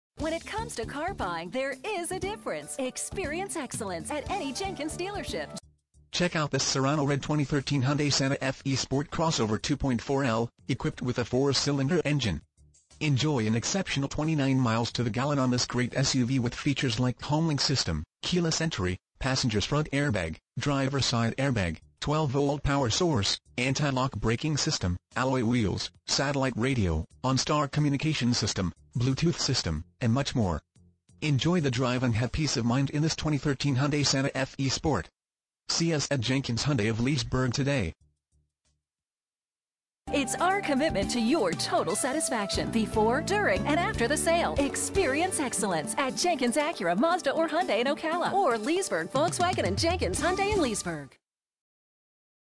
2013 Hyundai Santa Fe Sport Jenkins Hyundai Of Leesburg Leesburg, FL 34788
Hyundai Santa Fe Sport You will love this Serrano Red Hyundai Santa Fe Sport Crossover, equipped with a 4 Cylinder engine. Enjoy an exceptional 29 miles to the gallon on this great SUV with features like Satellite Radio, OnStar Communication System, Passenger's Front Airbag, Driver Side Air Bag, Anti-Lock Braking System, Homelink System, Bluetooth System, Alloy Wheels, Keyless Entry, 12 Volt Power Source, and much more. Enjoy the drive and have peace of mind in this Hyundai Santa Fe Sport. See us at Jenkins Hyundai Of Leesburg today!
MILES: 14,259
VIN:5XYZT3LB1DG041539

9145 US Hwy 441
Leesburg, Florida 34788